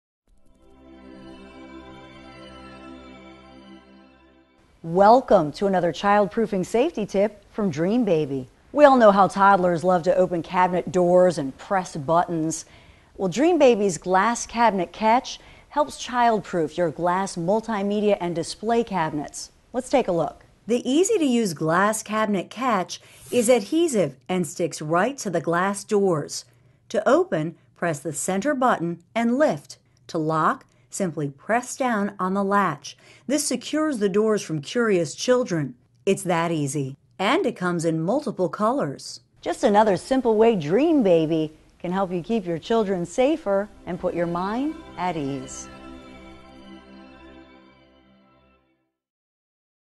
Pengaman Pintu Lemari DreamBaby Glass Cabinet catch
Pengaman pintu lemari Dreambaby Glass Cabinet Catch merupakan peralatan keamanan bayi yang terbuat dari plastik kuat dan bisa menahan pintu lemari/kabinet agar tidak mudah dibuka oleh si buah hati, sehingga meminimalisir rasa sakit akibat terjepit pintu lemari/kabinet. Dengan Dreambaby Glass Cabinet Catch, si buah hati pun tetap nyaman dan aman saat bermain dan beraktifitas saat di rumah.

Spesifikasi :
- Bisa digunakan untuk pintu lemari/kabinet kaca, kayu dan plastik
- Warna Putih
- Ukuran 10 cm (P) x 3,5 cm (L)
- Terdiri dari 1 pcs Dreambaby Glass Cabinet Catch dalam setiap paketnya